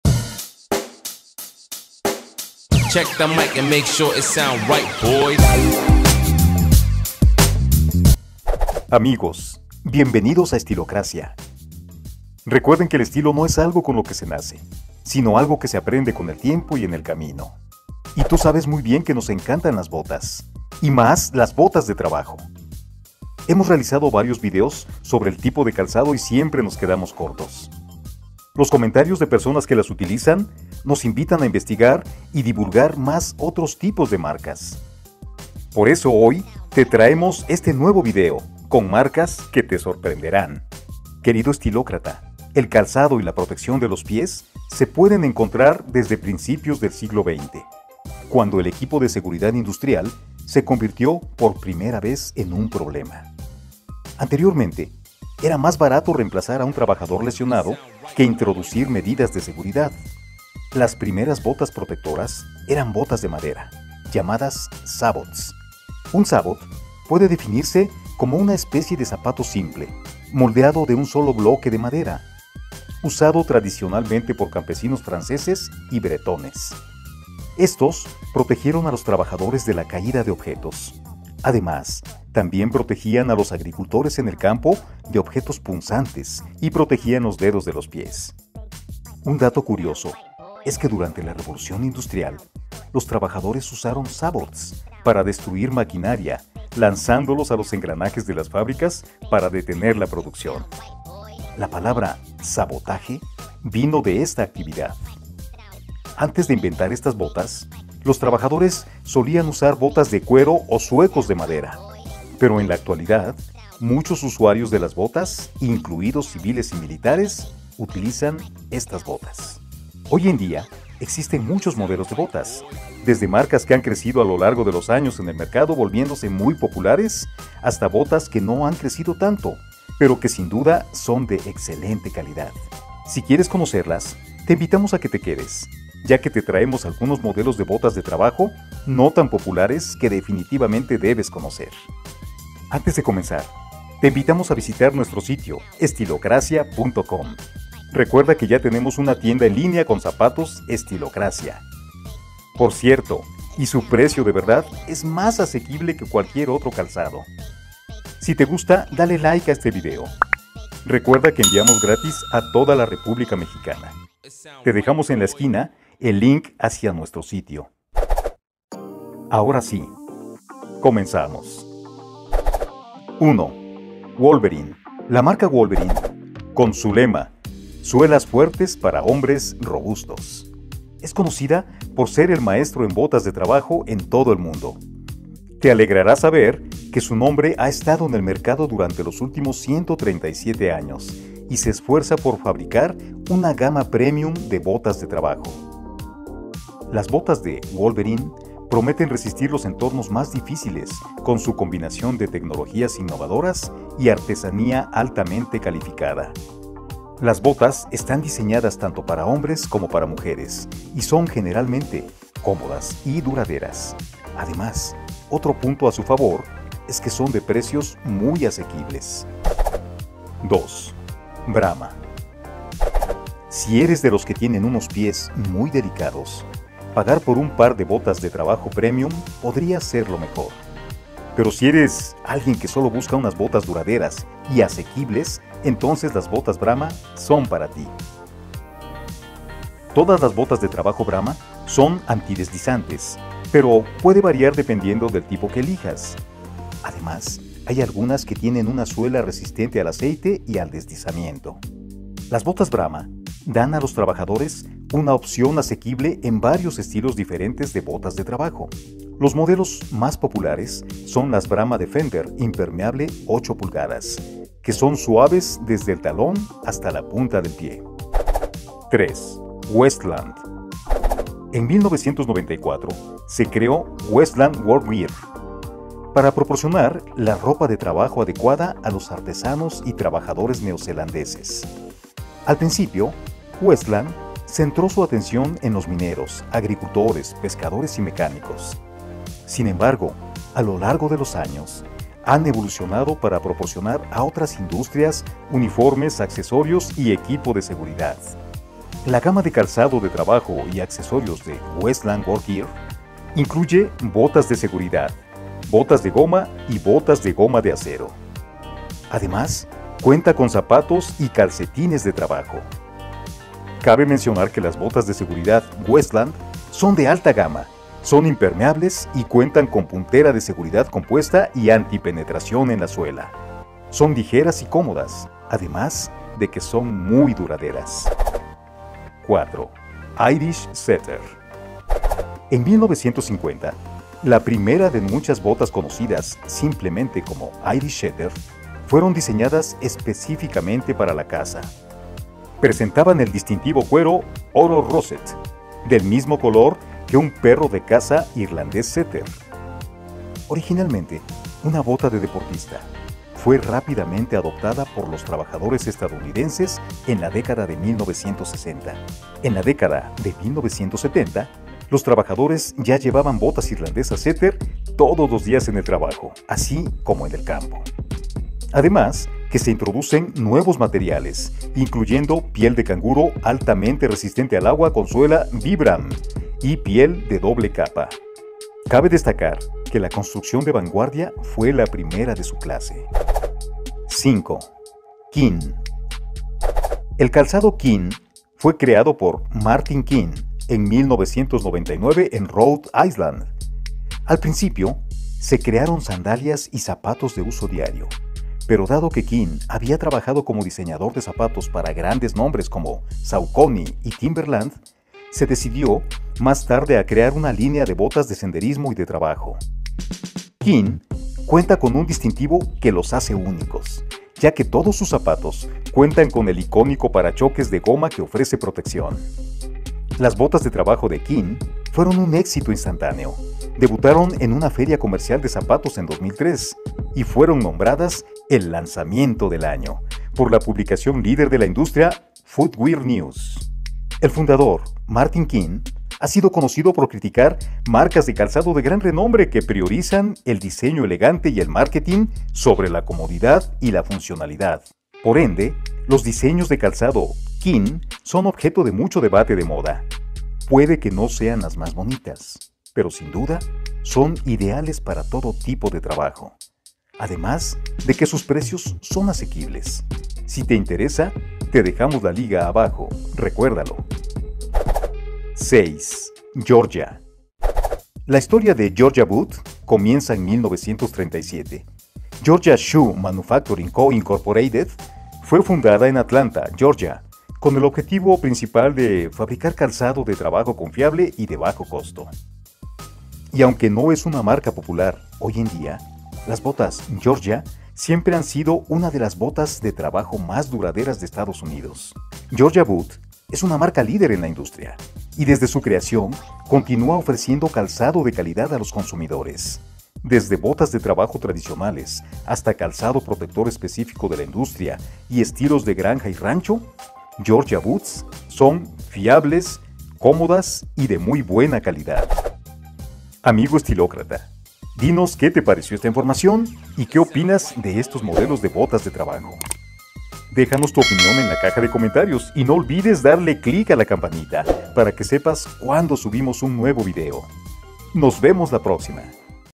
BOTAS DE TRABAJO que NO CONOCES!!!! [HOMBRE 2021]
HAY MUCHAS BOTAS DE TRABAJO, PERO ALGUNAS MARCAS SON MÁS FAMOSAS QUE OTRAS. TE ASEGURAMOS QUE NO CONOCES ESTAS MARCAS...

Visita nuestro sitio asociado de Amazon para productos de tu interés: